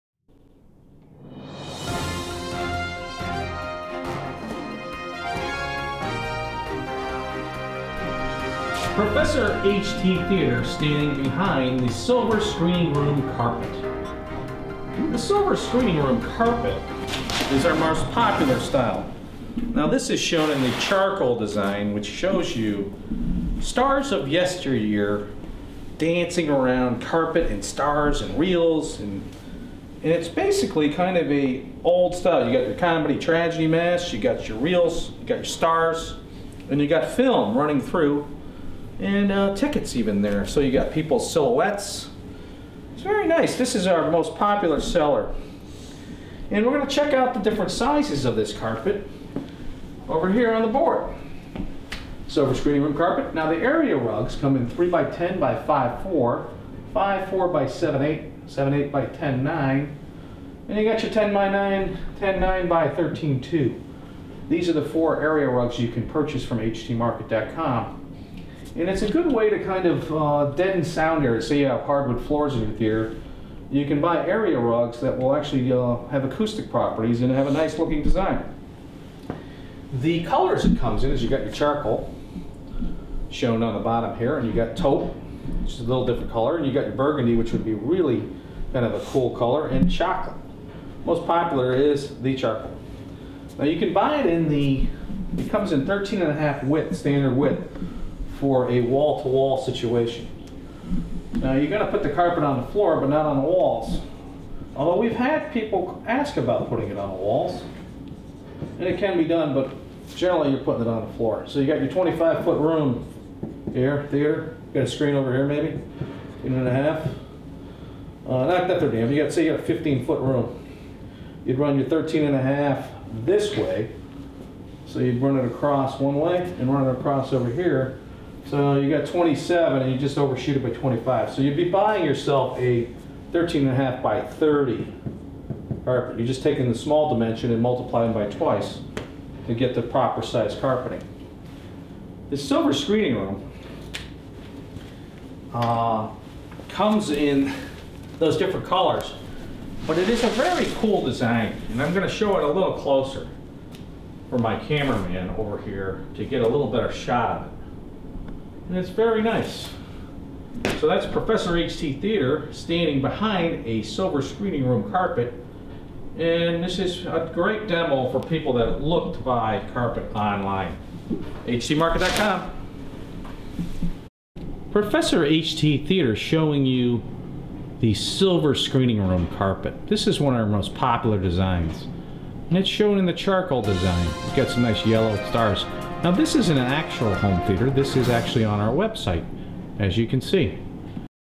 Silverscreen Home Theater Carpet at HTmarket.com
Silverscreen carpet for your home theater is the number 1 selling home theater carpet by HTmarket.com.  It has a classy design that brings out the roaring 20's and the golden age of cinema.  A home cinema is not complete without home theater carpet.